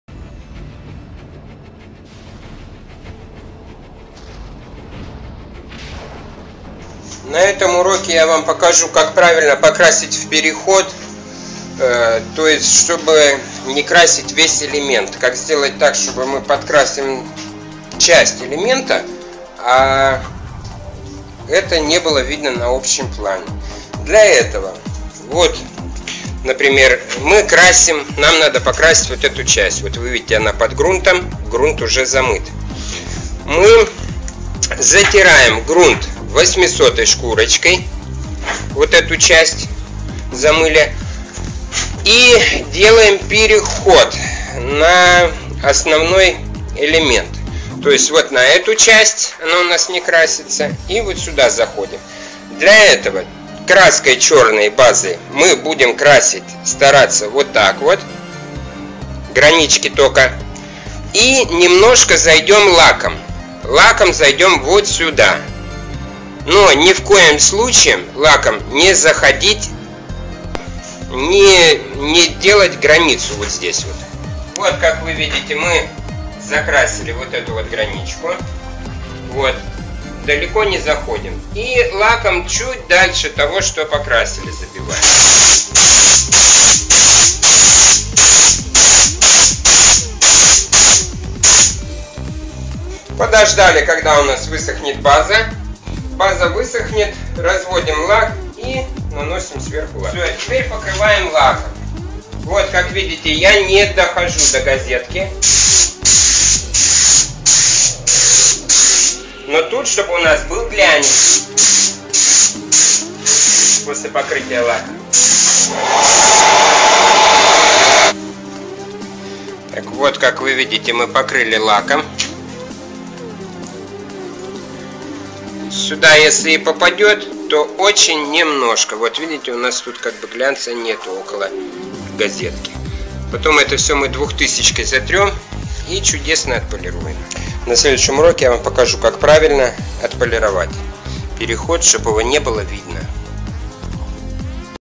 Как правильно покрасить авто переходом.Часть1
полируем переход ,красим переходом в двух частях